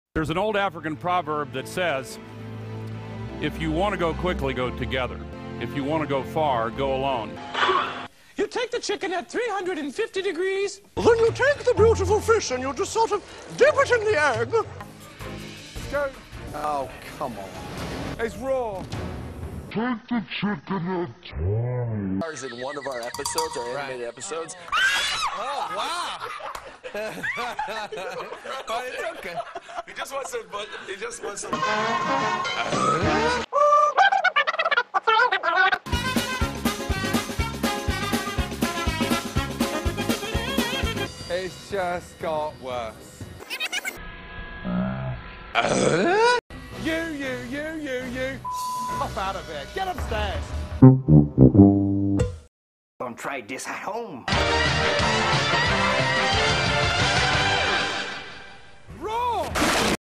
NXPQuickie#14: MARC SUMMERS COOKS RAW CHICKEN WITH KINKAJOU
Yep, I decided to throw together and upload a little teaser for my next episode of NXP, which I PROMISE IS COMING. I'm already over 5 minutes into it as I type. I literally did this in a little over an hour—not bragging, just saying. Enjoy it! :)